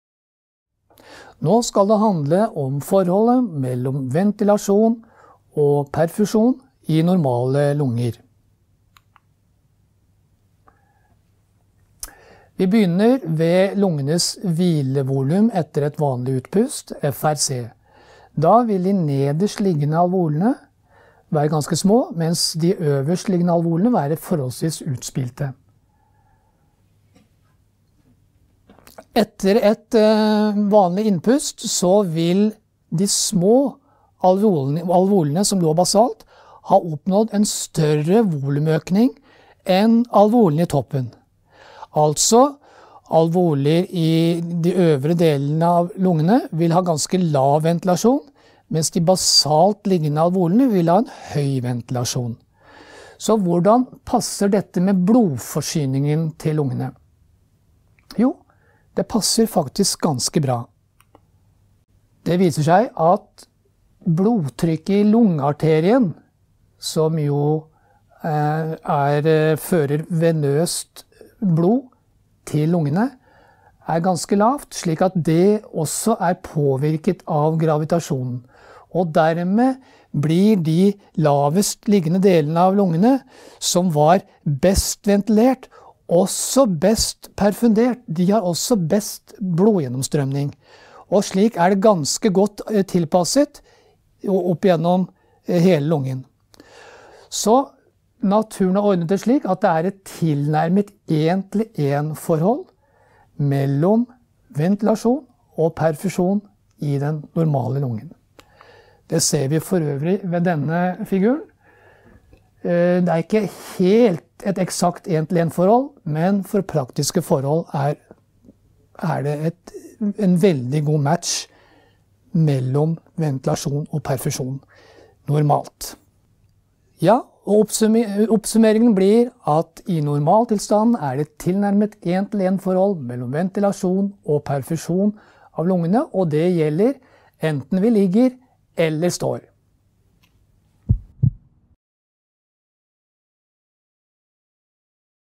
Ventilasjon og perfusjon
Miniforelesning laget for kurs i Lungefysiologi. Et samarbeid mellom HiOA, Universitetet i Tromsø og Norsk Fysioterapiforbund.